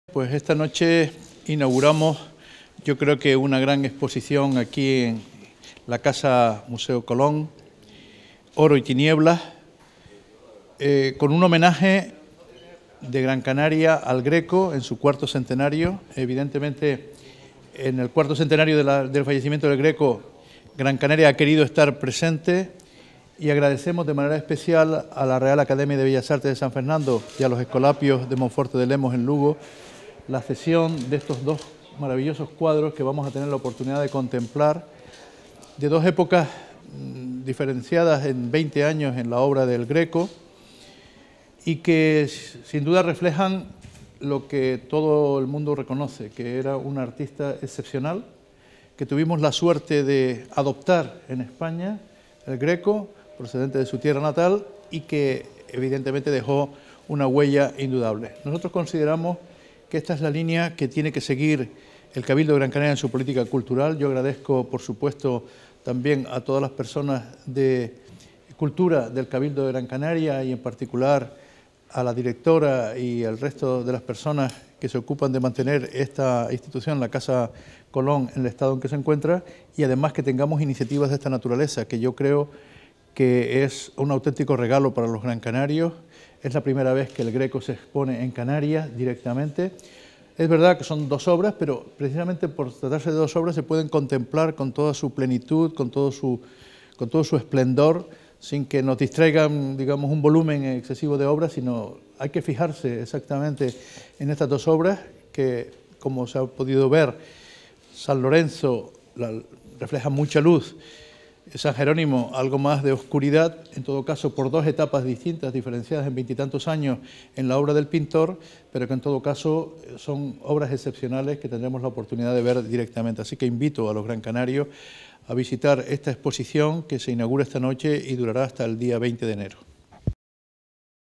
Exposición ‘El Greco: Oro y Tinieblas’ en Gran Canaria
Las Palmas de Gran Canaria | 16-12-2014

El presidente del Cabildo de Gran Canaria, José Miguel Bravo de Laguna, presentó este martes a los medios de comunicación la muestra ‘El Greco: Oro y Tinieblas’, que se inaugura hoy, a las 20.00 horas, en la Casa de Colón, donde permanecerá abierta, hasta el próximo 20 de enero.

La exposición, que contará con visitas guiadas gratuitas (previa inscripción) de lunes a viernes, a las 18.30 y a las 19.30 horas, incluye las primeras obras del pintor cretense, máximo exponente del Manierismo, llegadas nunca a Canarias: ‘San Jerónimo penitente’ (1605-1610) y ‘Aparición de la Virgen a San Lorenzo’ (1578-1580), provenientes de las colecciones de la Real Academia de Bellas Artes de San Fernando (Madrid) y del Colegio de Nuestra Señora de la Antigua de Monforte de Lemos (Lugo), respectivamente.

Con esta muestra, Gran Canaria se suma a la conmemoración del IV Centenario de la muerte de El Greco, lo que “además de constituir un notable acontecimiento cultural que a buen seguro entusiasmará a los admiradores de la pintura, supone un regalo para todos los grancanarios en estas fechas navideñas”, según el mandatario insular.

El pintor, llamado inicialmente a plasmar iconos bizantinos en su Creta natal y que alcanzaría su plenitud expresionista en España, inmortalizando el fervor religioso aprendido de maestros italianos como Tiziano y Tintoretto, muestra en las piezas escogidas para la muestra la esencia de su genio, así como la técnica pictórica, extraordinariamente depurada, por la que tanto ha influido en las sucesivas corrientes del arte moderno.

El empeño del Cabildo grancanario por acercar a la ciudadanía obras como éstas, representativas de grandes maestros de la Historia del Arte, no es nuevo. Hace unos meses se trasladaba a Gran Canaria una importante selección de cuadros del pintor valenciano Joaquín Sorolla, que estuvo alojada en las dependencias de San Martín Centro de Cultura Contemporánea, y poco antes llegaba, también a la Casa de Colón, un ‘San Andrés’ de José de Ribera.

“Ese esfuerzo seguirá plasmándose próximamente con la exhibición de nuevas piezas, merced a acuerdos y convenios con instituciones como la Real Academia de Bellas Artes de San Fernando”, con la que, avanzó Bravo de Laguna, “se han establecido nuevos lazos al hilo de esta exposición que servirán para que, en adelante, recalen en Gran Canaria obras de tanta o similar calidad como la que tienen estos lienzos de El Greco”.

Al acto asistieron también el coordinador general de Cultura, Patrimonio Histórico y Museos del Cabildo, Larry Álvarez, el comisario de la muestra, José Luis Gago, y el miembro de la Real Academia de Bellas Artes de San Fernando Juan Bordes, que mañana miércoles, 17 de diciembre, a las 20.00 horas, ofrecerá la conferencia ‘Mi Greco’, también en el museo americanista.

Larry Álvarez recordó por su parte que esta muestra supone probablemente “el primer gran hito cultural en la isla tras el anuncio del Cabildo, hace solo unas semanas, de la convocatoria de un concurso de ideas para el futuro Museo de Bellas Artes de Gran Canaria en el antiguo Hospital de San Martín”, pero, hasta la apertura del MABGC, la Casa de Colón seguirá exhibiendo exposiciones temporales que, como la que hoy se inaugura, “servirán para complementar su colección permanente y, en todo caso, para enriquecer la oferta cultural insular”.

‘San Jerónimo’

El Greco pintó a San Jerónimo en tres versiones: como cardenal, penitente de cuerpo entero y penitente de medio cuerpo. La obra de la Academia pertenece al tercer grupo, siendo uno de los de mayor interés artístico. El retrato transmite con destreza la vida eremítica del santo, cuyo cuerpo se muestra silueteado en medio de una tenebrosa cueva, y el misticismo del personaje, representado con la piedra con la que éste se golpea el pecho, su mirada hacia el crucificado y la presencia de una calavera junto a un reloj de arena.

‘Aparición de la Virgen a San Lorenzo’

La pintura de Monforte de Lemos es, por el contrario una pieza única: El Greco no hizo ninguna otra representación de San Lorenzo, pese a la proximidad del tema a las obras de El Escorial y, por tanto, al interés personal de Felipe II. Considerada como de la primera etapa toledana, la obra muestra a un joven resignado que enseña a la Virgen el objeto de su martirio. Sorprenden los detalles de la preciosa dalmática que viste el santo, probablemente inspirada en la belleza de los tejidos empleados de la sede arzobispal de Toledo, pero, sobre todo, la influencia formal y cromática del cielo, con el que el artista logra combinar a la perfección las partes en que se divide la escena.

ACFI PRESS